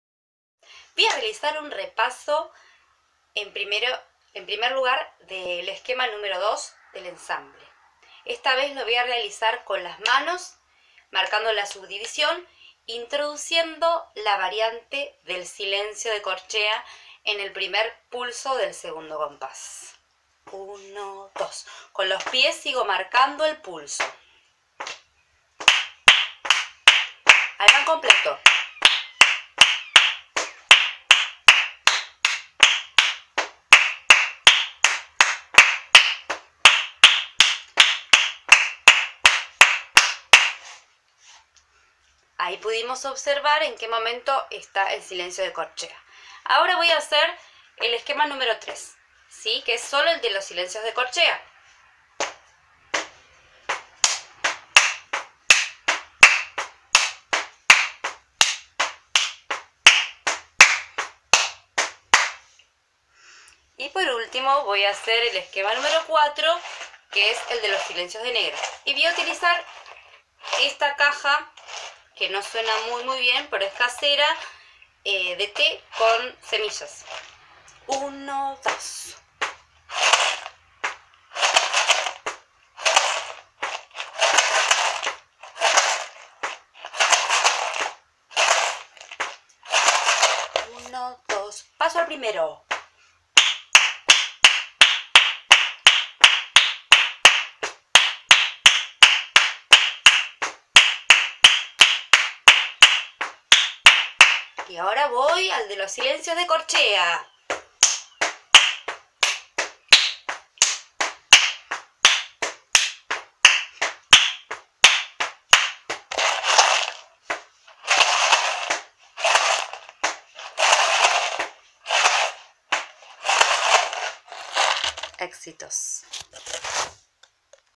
16 - Esquemas del ensamble - revisión con palmas
Aqui realizaremos la misma lectura rítmica; esquema por esquema; con palmas. PUES EL MOTIVO ES REALIZAR LA PRÁCTICA; DIFERENCIACIÓN E IDENTIFICACIÓN DE LOS SILENCIOS DE CORCHEA Y DE NEGRA. De todas las formas que lo haríamos vívido; en este video se realiza de manera sencilla: solamente con nuestro cuerpo.

Lectura rítmica nro 1 original para ensamble - hoja 3 del pdf
(con variante en el esquema nro 2 - correspondiente a la subdivisión) (introducción del silencio de corchea nuevamente)

EXITOS!!

Prof Carolina I. Posadas
EMJA